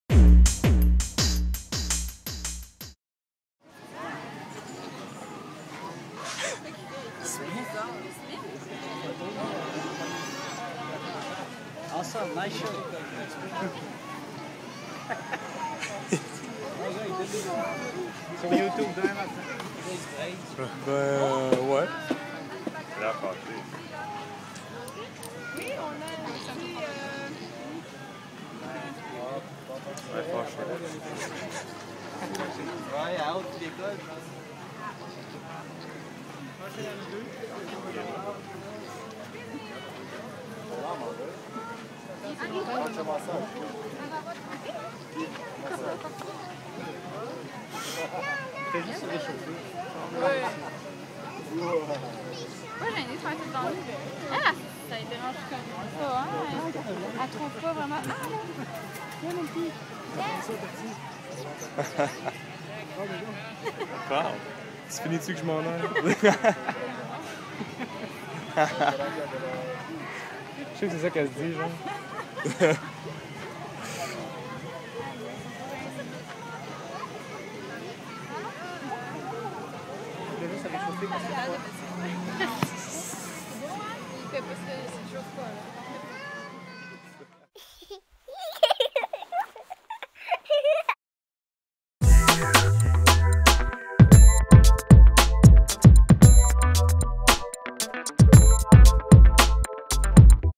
Two lamas having s3x!
Two lamas having fun at the Granby Zoo!